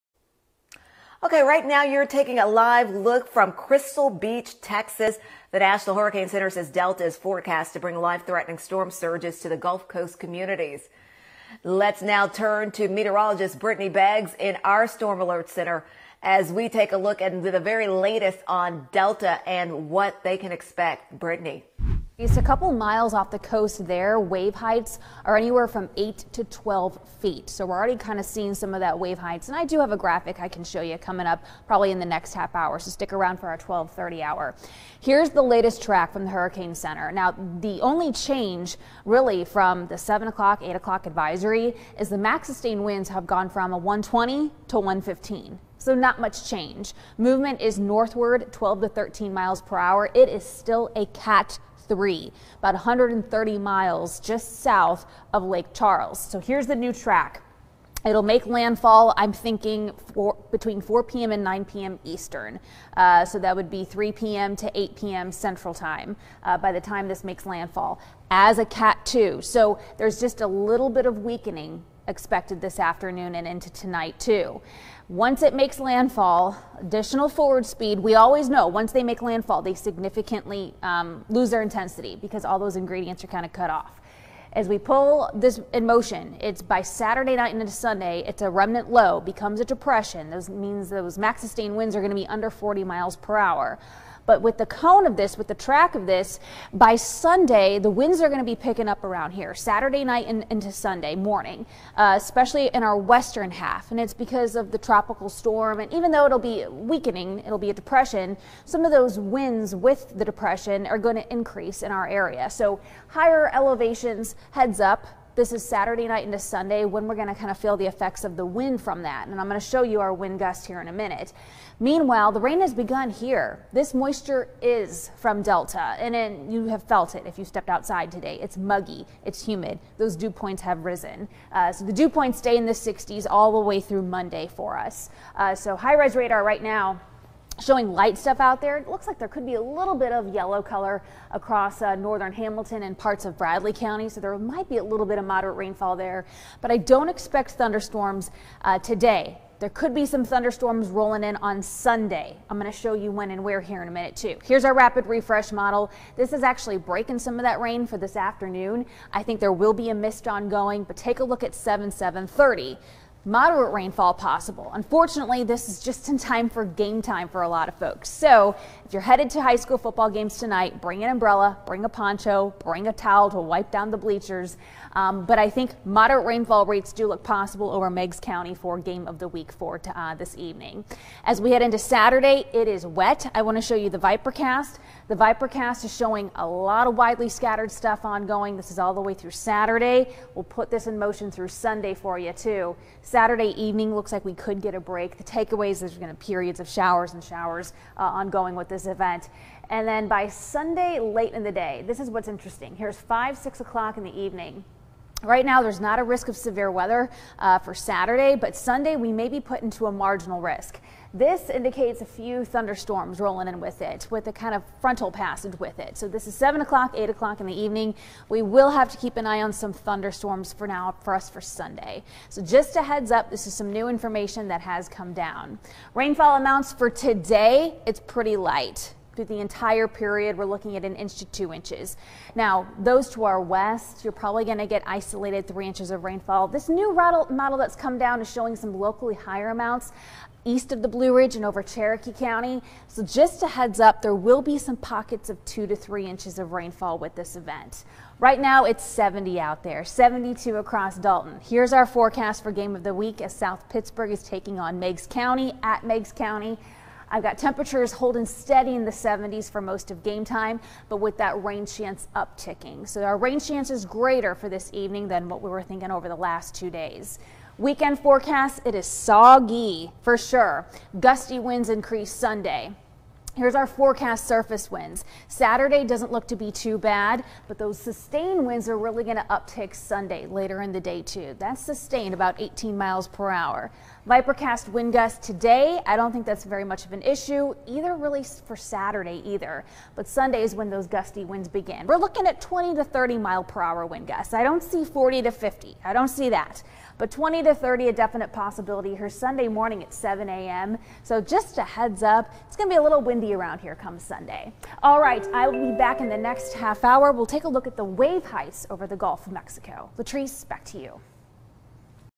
Brittany Beggs' Friday forecast
Showers will increase overnight and continue Saturday and Sunday with periods of heavy rainfall.